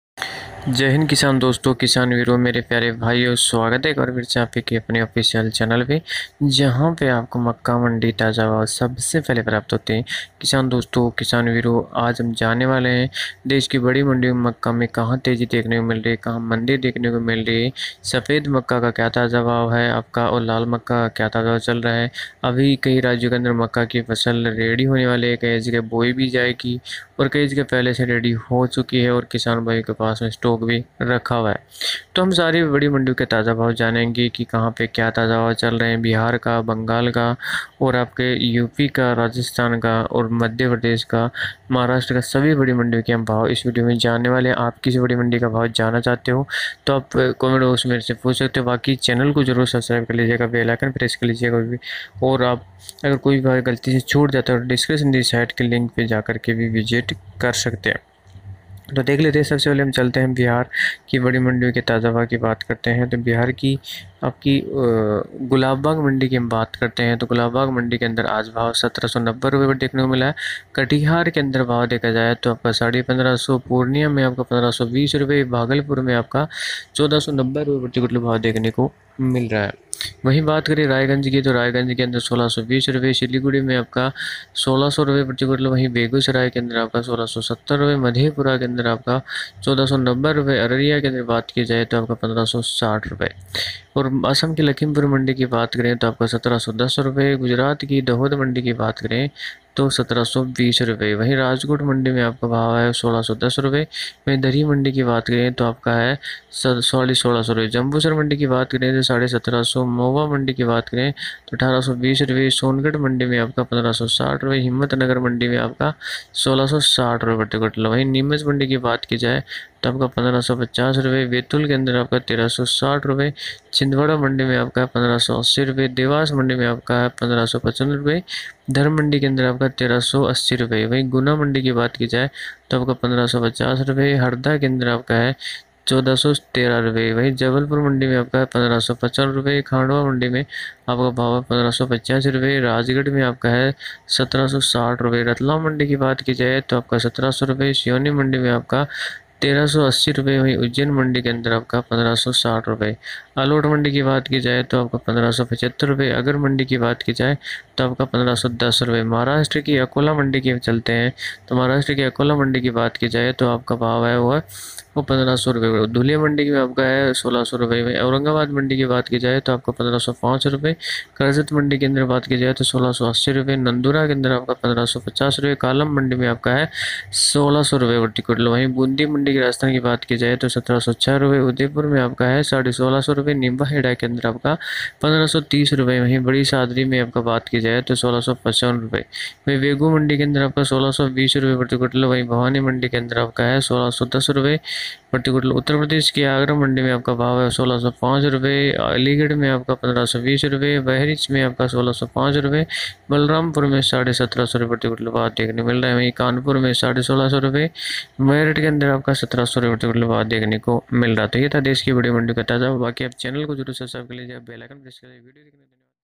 14 सितम्बर 2021 मक्का मंडी ताजा भाव, मक्का भाव में भारी तेजी,maize bhav today,makka bhav,mandi bhav
0देश की बड़ी मंडियों के मक्का का भाव देखने के लिए यहां क्लिक करें